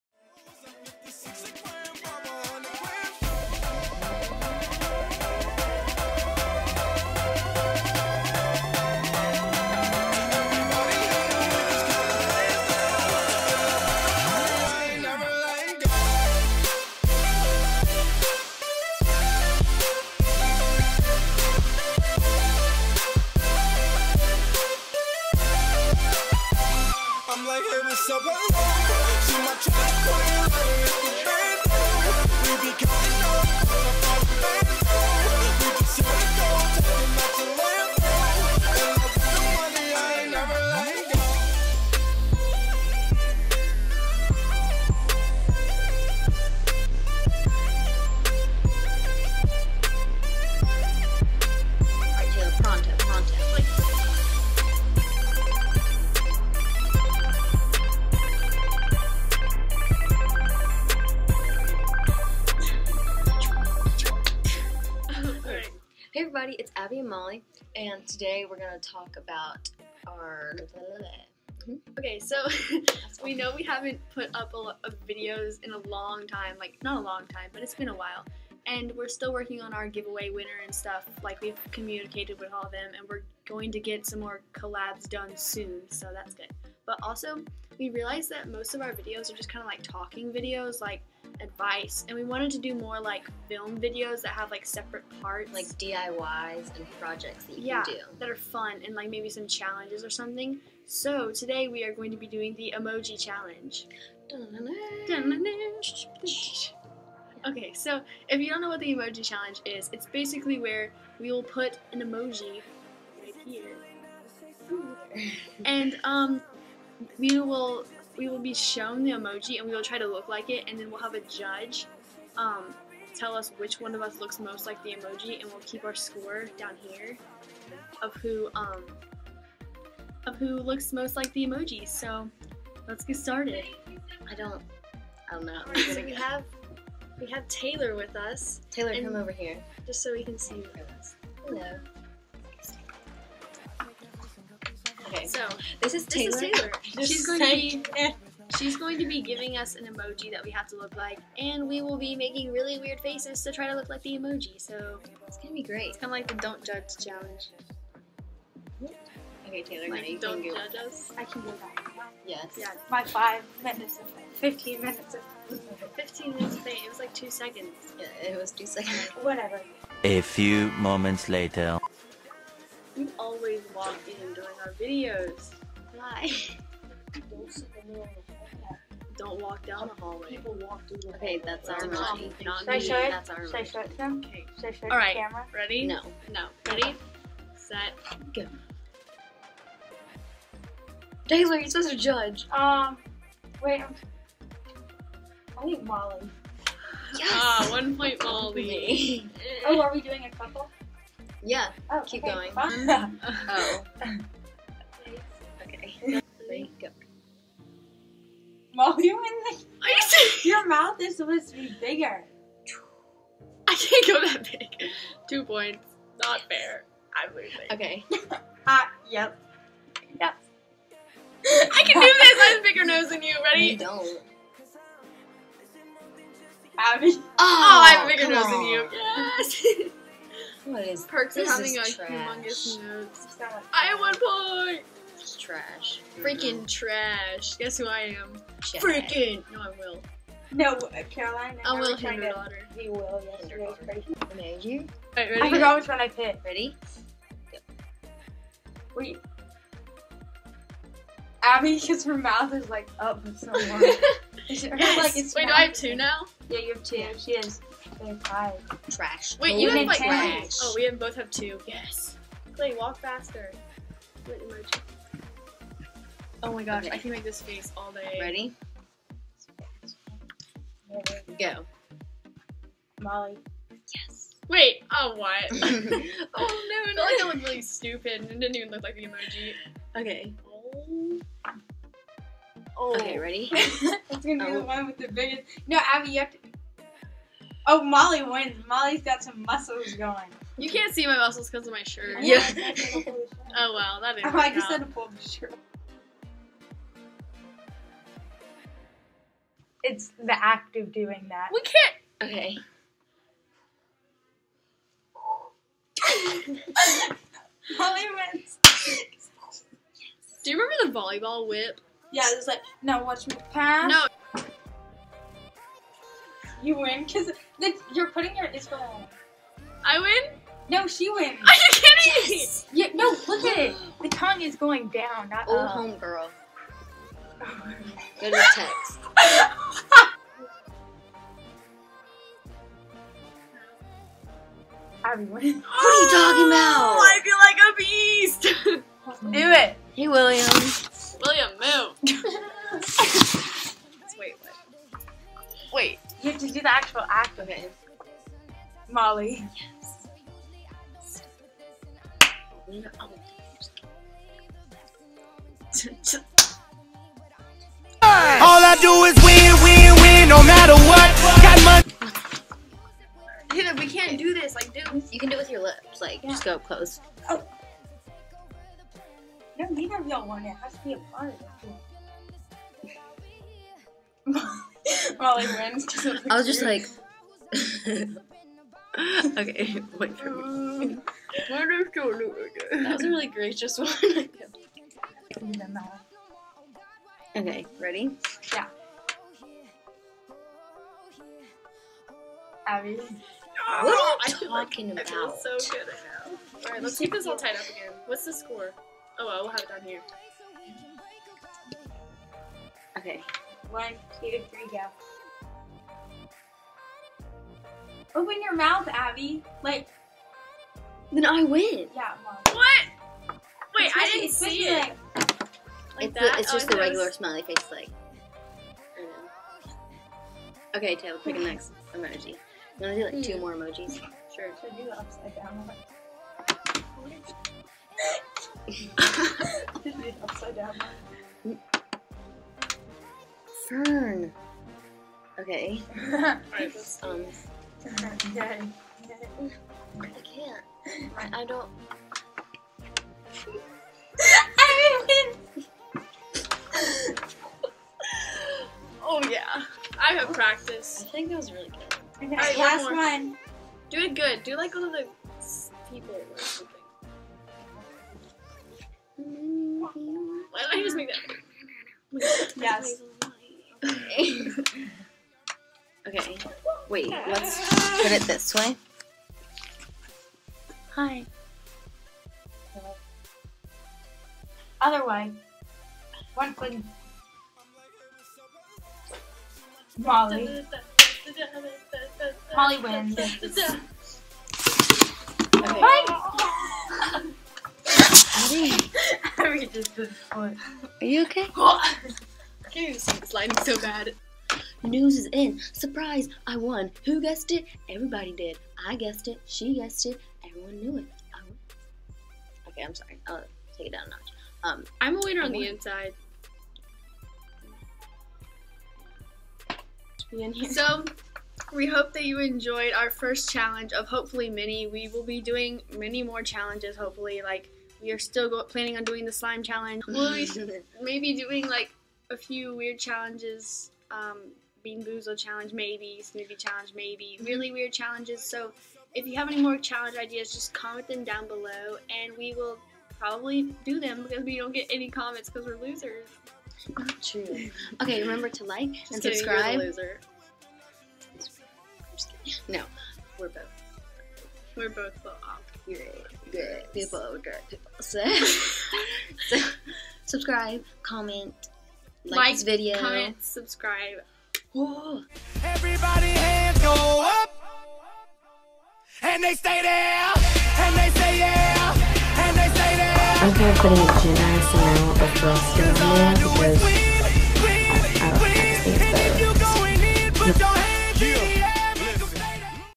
Emoji Challenge | Abi&Molly
Hey! This is the Emoji Challenge! I first saw this video on Devin Hayes's channel with Reed Deming, Sam and Colby. Check it out!
Thank you! More challenges on the way!